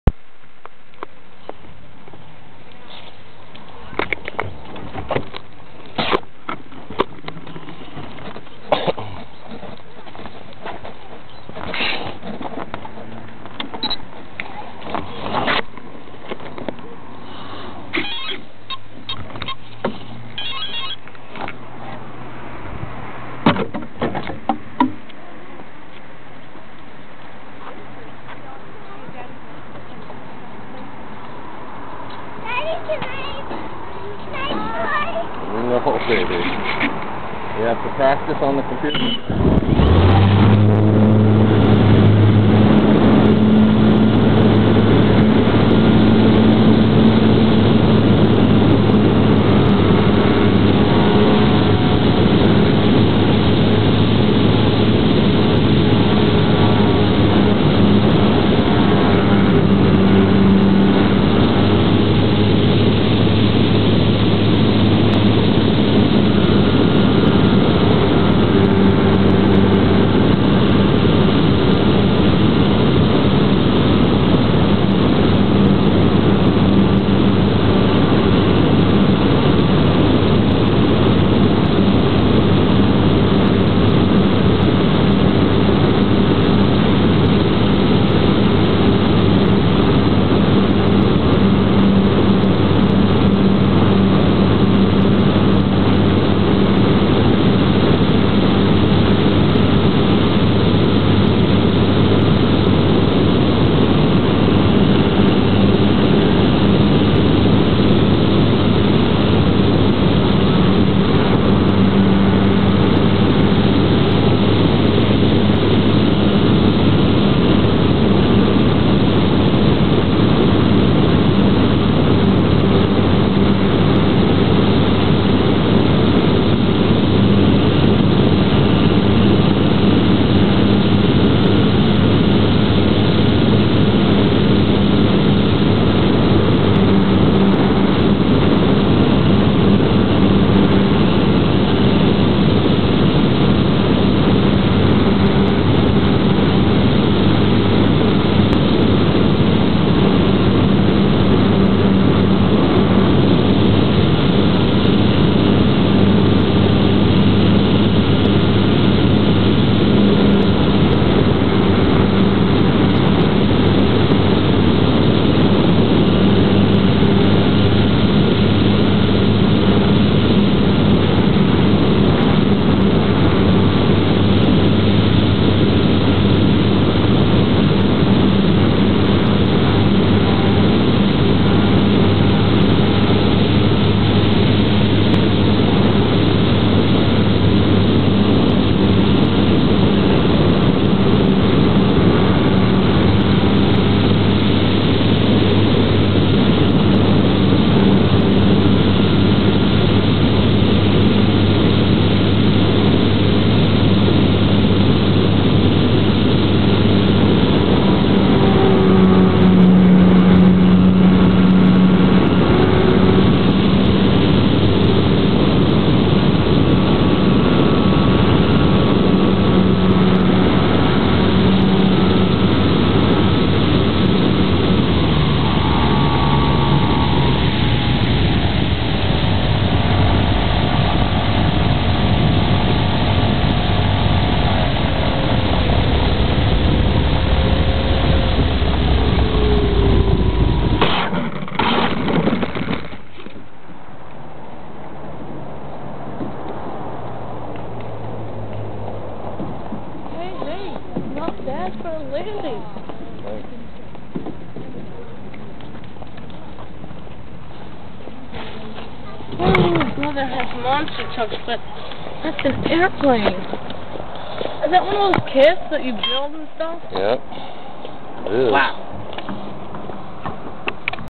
Slow Stick Over Redding CA 2
GWS Slow Stick over Redding CA Park.  This is a test flight with a brushless slowstick over Cascade Park in south Redding.